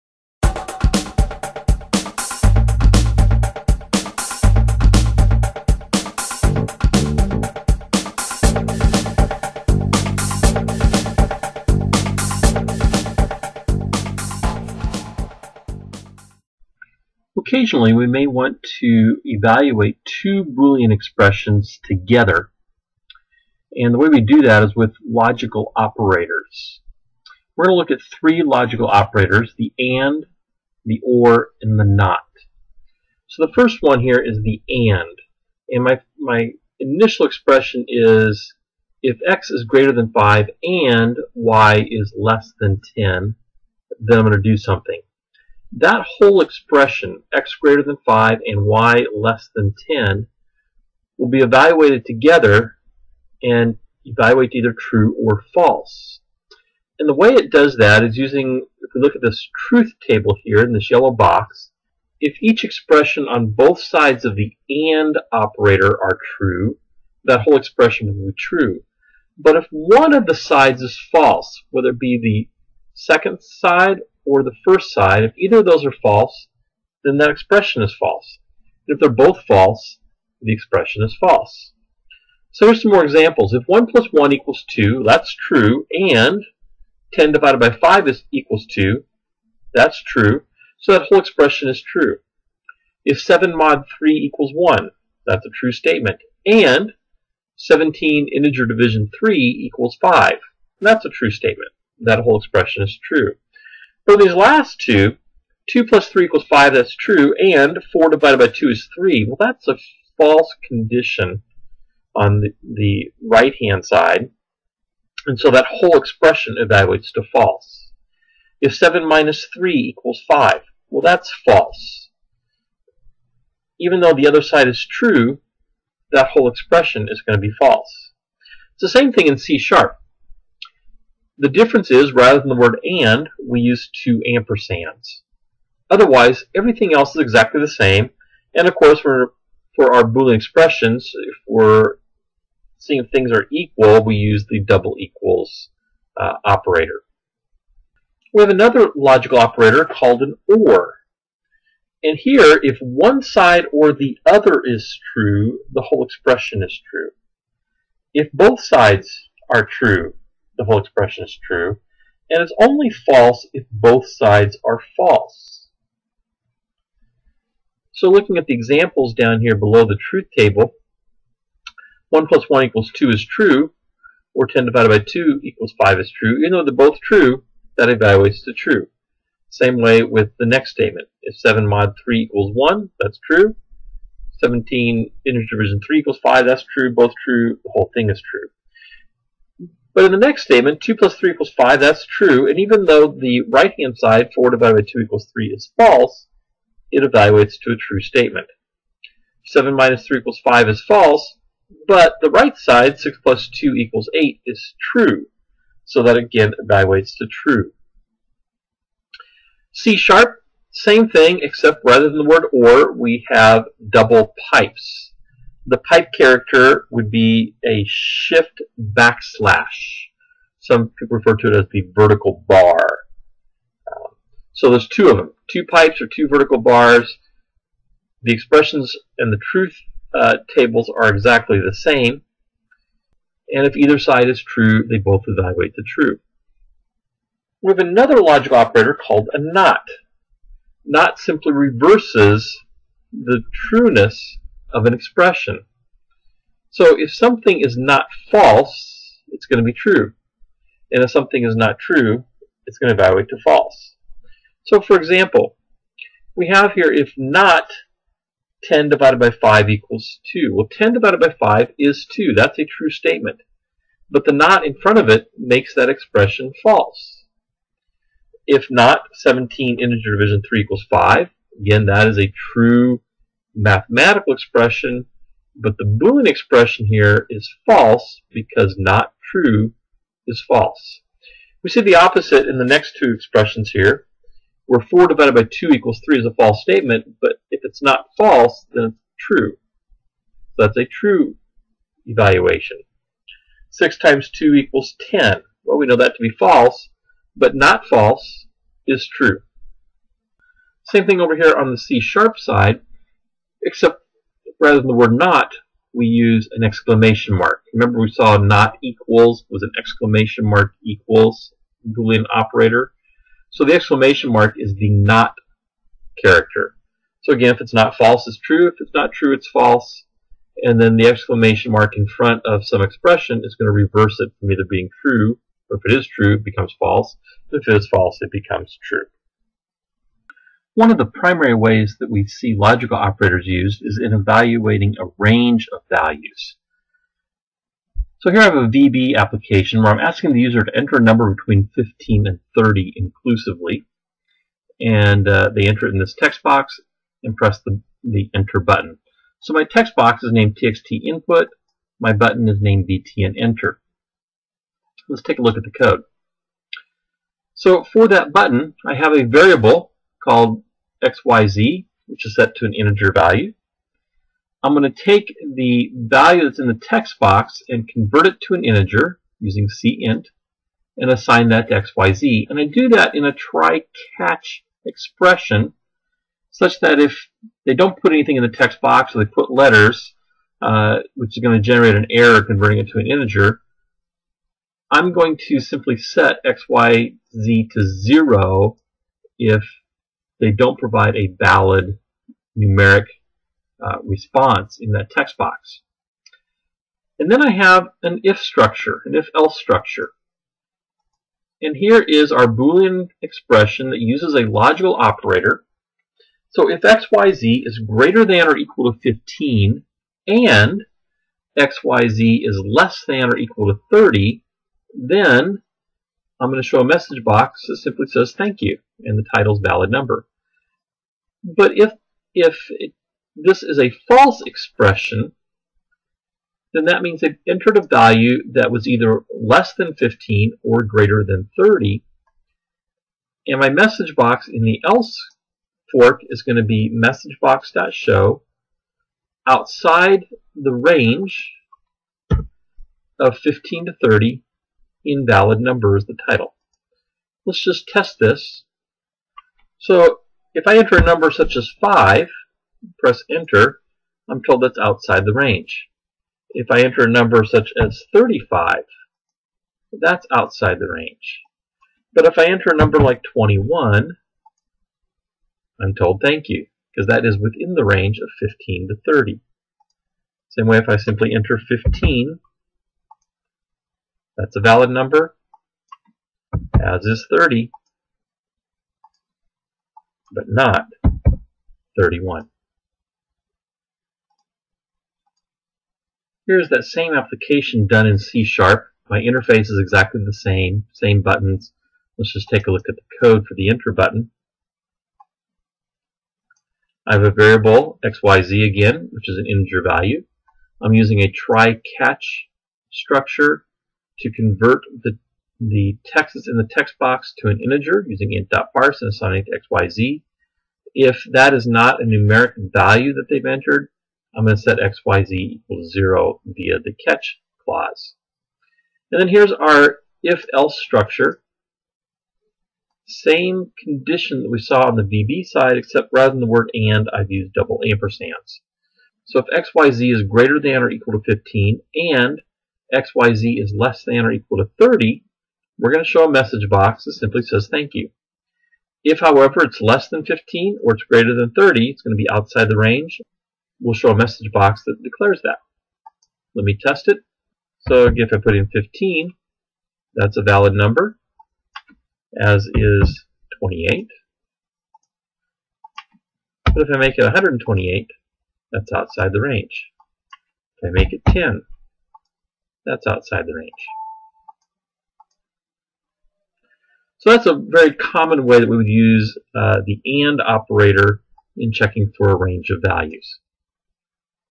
05J Logical Operators (VB & C#)
Microsoft Programming Lab-Based Learning class at South Mountain Community College: Unit 5 Video 10 -- Logical Operators (VB & C#) [Use of And, Or, Not in VB; Use of &&, ||, ! iin C# to create more complex Boolean expressions] Instructor: Stephen F. Hustedde